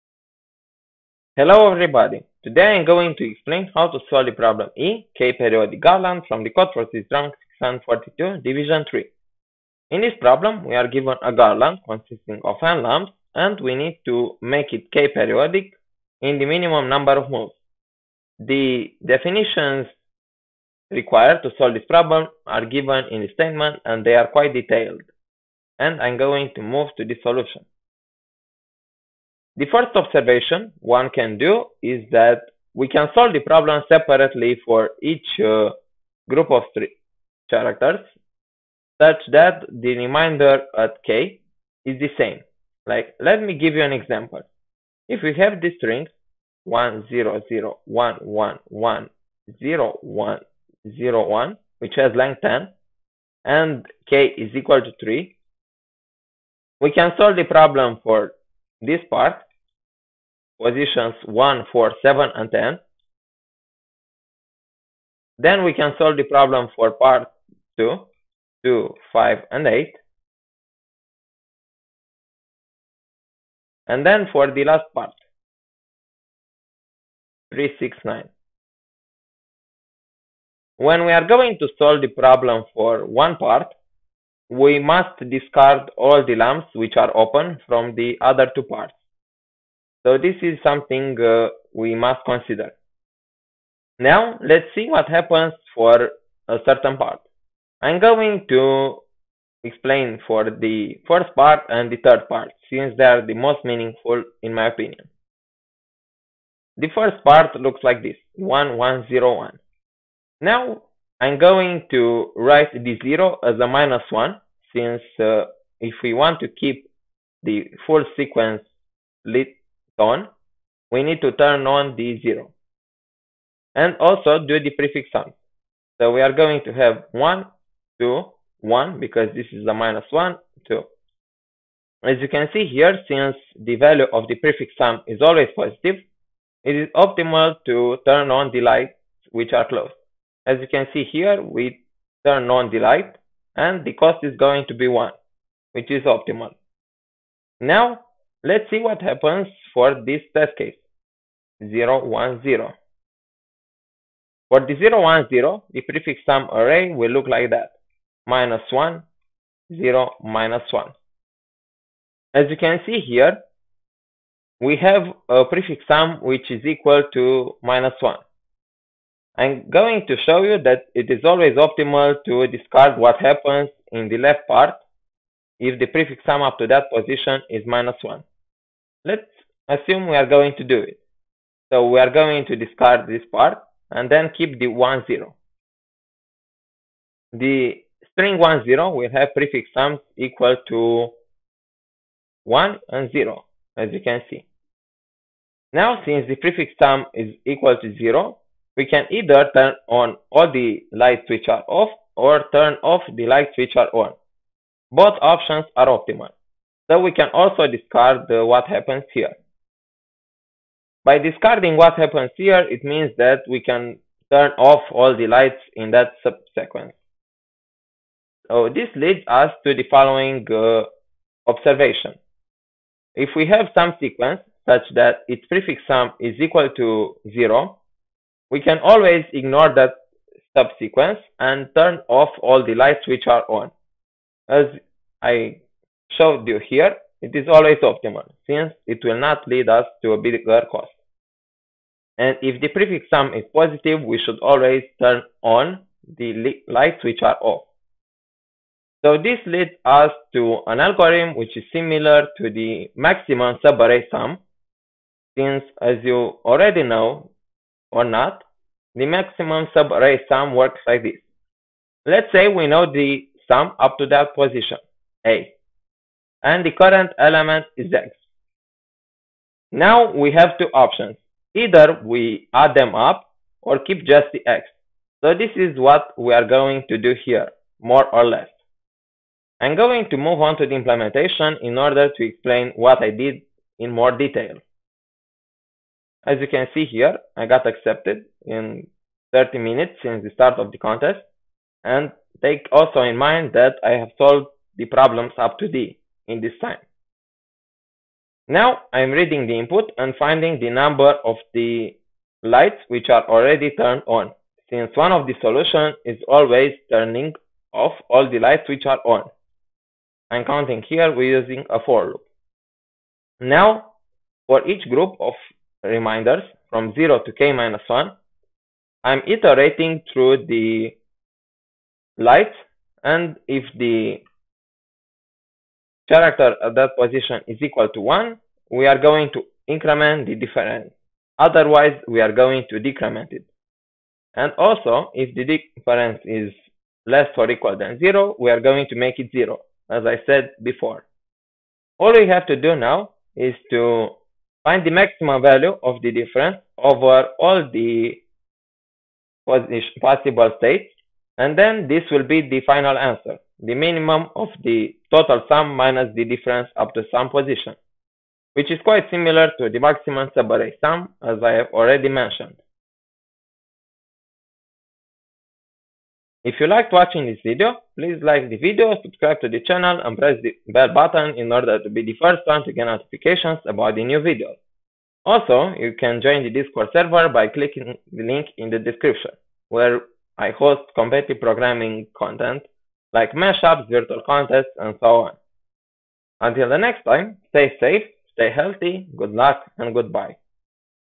Codeforces Round #642 (Div. 3) - E. K-periodic Garland - Video Tutorial
Today I'm going to explain E from Codeforces Round #642, a task which can be solved if we reduce the problem to something similar to max subarray sum